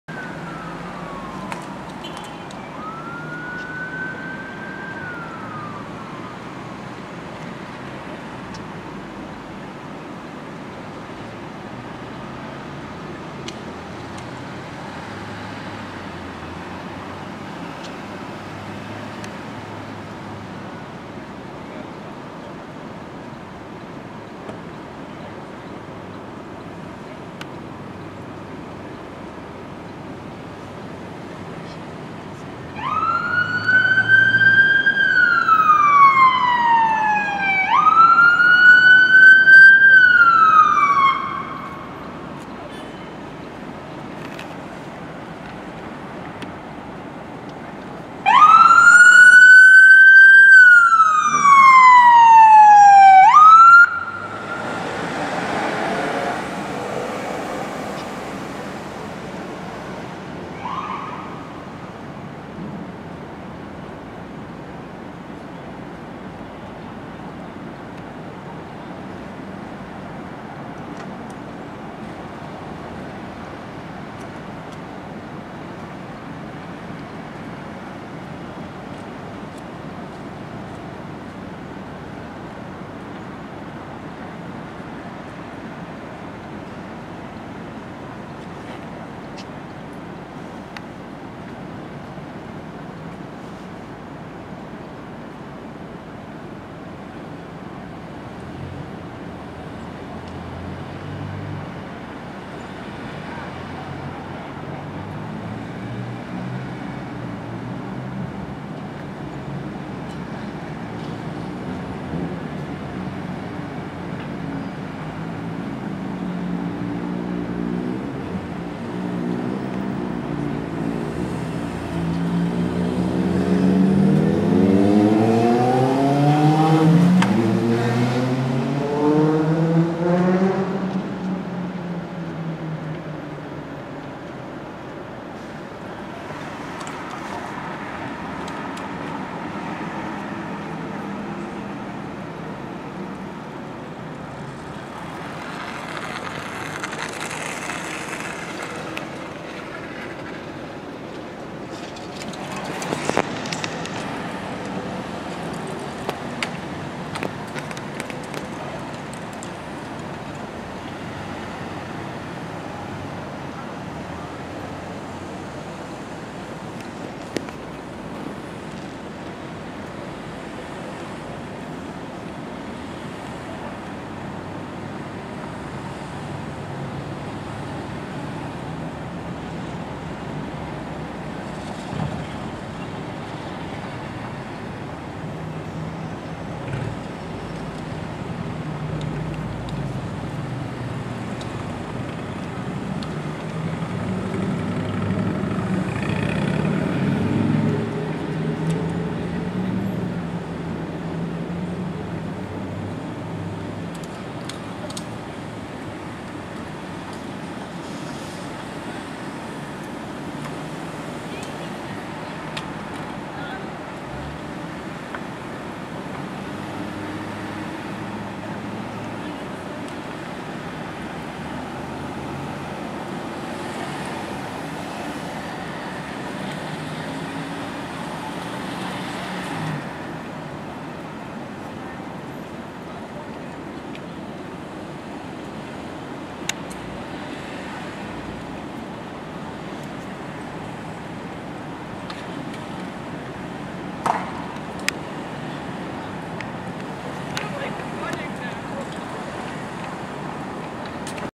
Oshawa Car accident with police cruiser.
Car accident in Oshawa